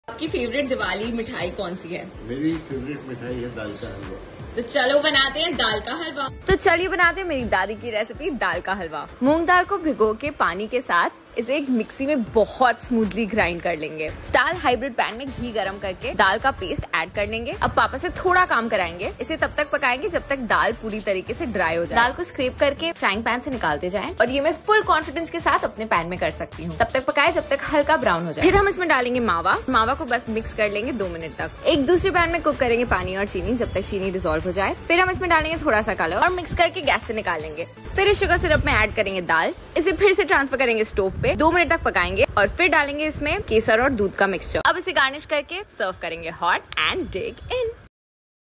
StahlRecipes - Dal ka Halwa in Stahl Hybrid Frypan
Nutritious Dal ka Halwa in Stahl Hybrid Frypan

Our favourite Blogger, Sneha Singhi Upadhaya, holds this recipe very close to her heart. This was the last thing her dadi taught her how to make, and she made it the best! There is no Diwali without Dal ka Halwa! ♥️

Sneha has used the Stahl Hybrid Frypan for the preparation of this delicious treat.

Serves 4

Ingredients:
🍯 ½ cup Mawa
🍯 ½ cup Water
🍯 ¼ cup + 2 tbsp Grain Sugar
🍯 6 Cardamom Seeds, Ground Saffron
🍯 ¼ tsp Yellow Food Colouring, mixed with 1 tsp Water/ Milk
🍯 Almonds and Pistachios, soaked and sliced

Method:
1. Soak moong dal in water for a couple of hours. Blend it into a smooth paste with some water in a blender.
2. Heat ghee in a pan & add in the dal paste. Keep cooking & scraping the pan till the dal dries up & browns a little. Add in the mawa, mix it well & cook for 2 minutes.
3. Heat water in a pan. Add in the sugar & mix till the sugar dissolves. Add the food colour.
4. Add the dried dal to the sugar syrup & mix well. Put it back on the heat & add saffron soaked in milk. Cook for 2-3 minutes. Add the cardamom powder. Garnish with dry fruits. Serve hot.